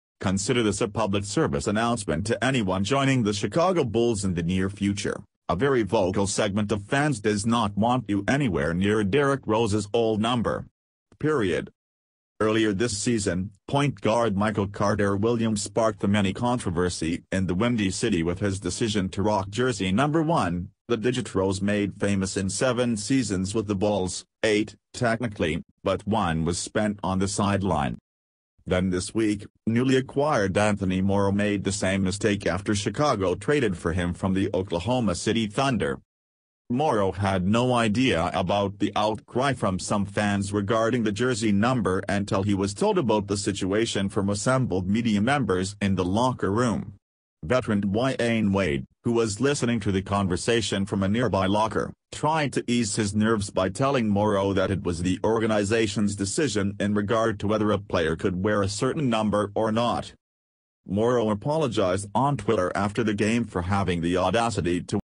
New Bulls player sparks controversy by wearing Derrick Rose’s old number
Morrow had no idea about the outcry from some fans regarding the jersey number until he was told about the situation from assembled media members in the locker room. Veteran Dwyane Wade, who was listening to the conversation from a nearby locker, tried to ease his nerves by telling Morrow that it was the organization’s decision in regard to whether a player could wear a certain number or not.